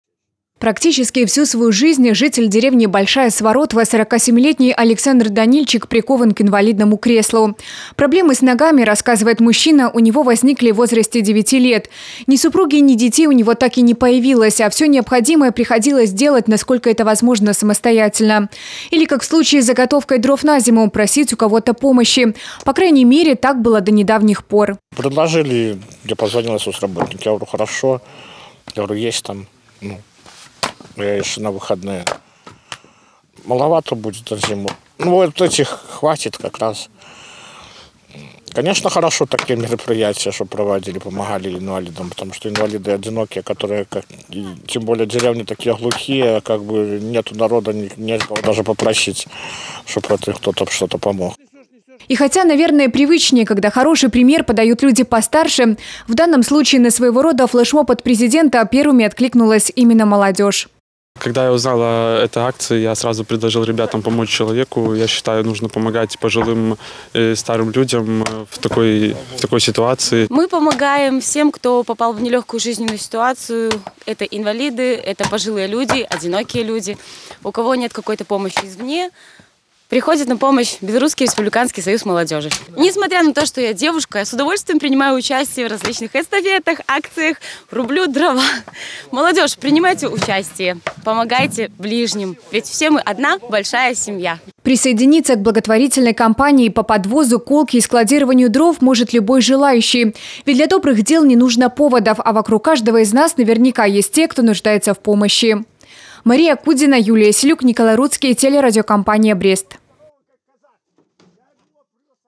ЭСТАФЕТА ТЕПЛА
«Теплую» эстафету, несмотря на достаточно прохладную погоду за окном, подхватили активисты БРСМ Барановичского района. Тренд по колке дров, напомним, запустил Глава государства. Молодежь не осталась в стороне и отправилась помогать тем, кто нуждается в помощи.